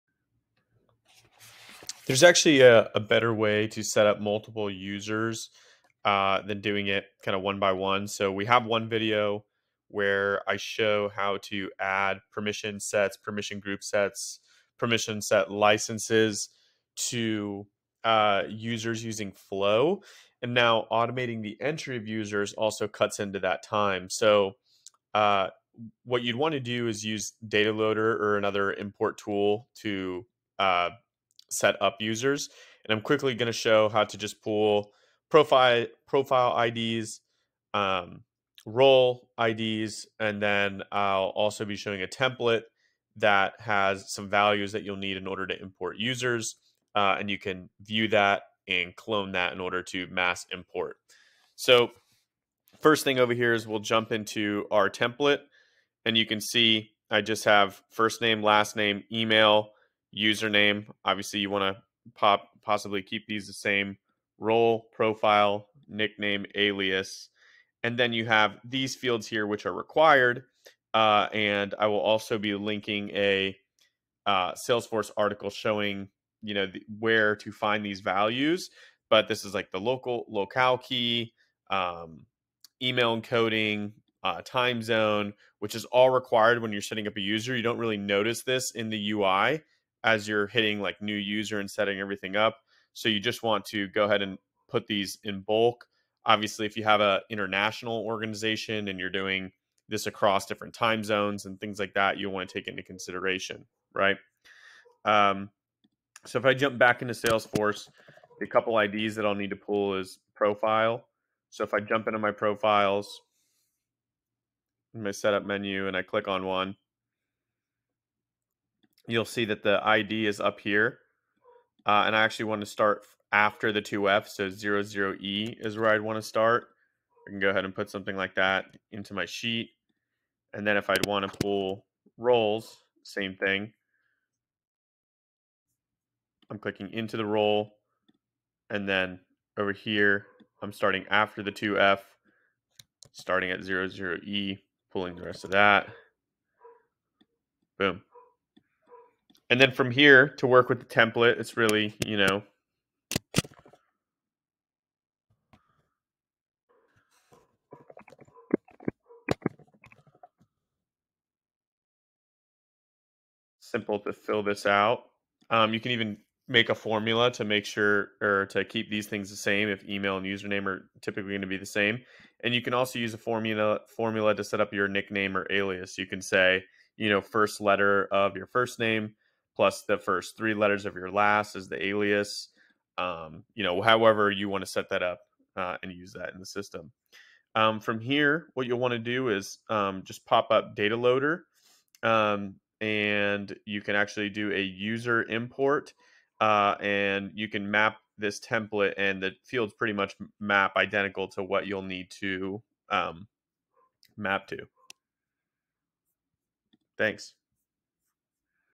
Salesforce Mass Importing Users: How to find IDs and build a Template
Quick video on how to find role/Profile Ids and also prep for a user import. Dataloader is a free tool and works well with the template.